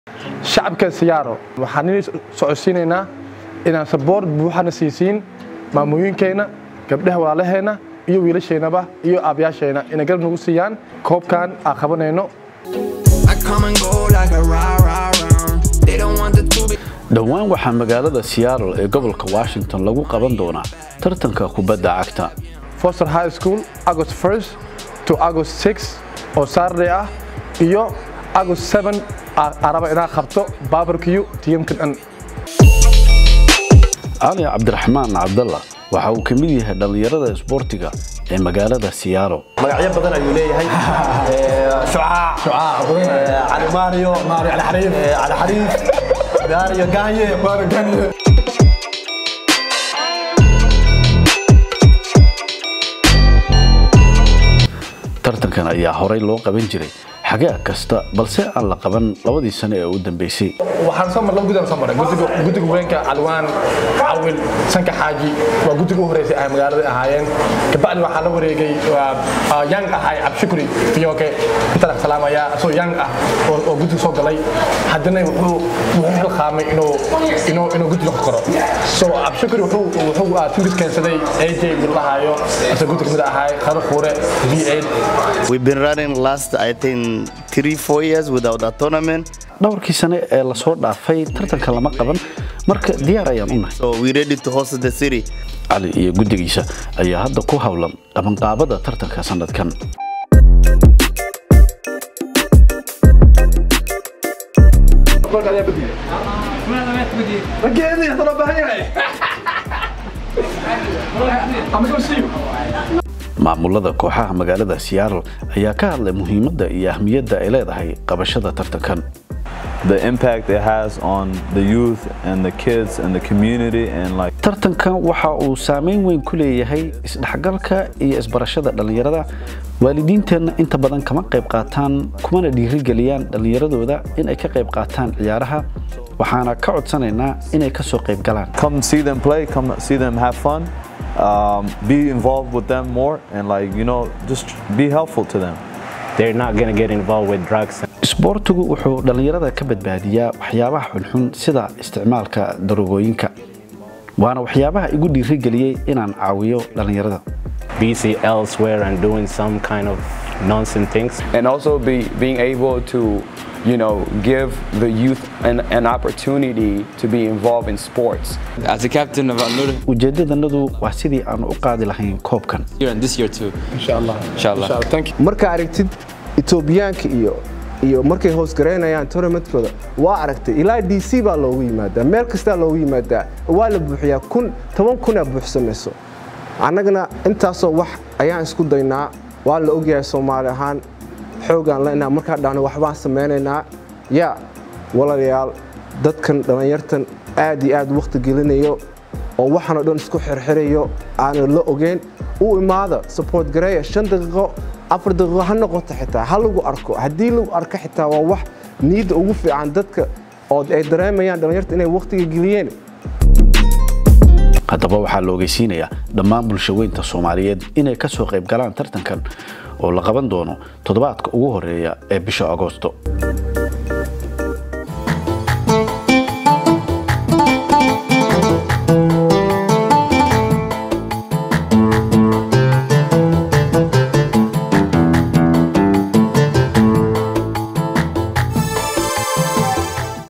Koobka Seattle 2021 | Seattle Somali Soccer Tournament | Somali Bridge - USA
The Seattle Somali soccer tournament will be hosted on August 1-7th, 2021 at Foster High School.

Koobka kubadda cagta waxaa sannadkan lagu qabanayaa bisha Agoosto 1da ilaa 7da iyo Foster High School